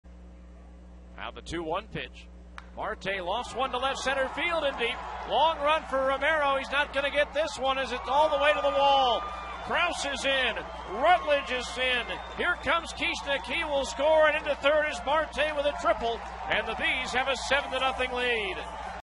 Salt Lake's Marte drives in three runs
5/7/15: Salt Lake's Alfredo Marte drives in three runs with a triple in the third inning of a 10-3 win over Tacoma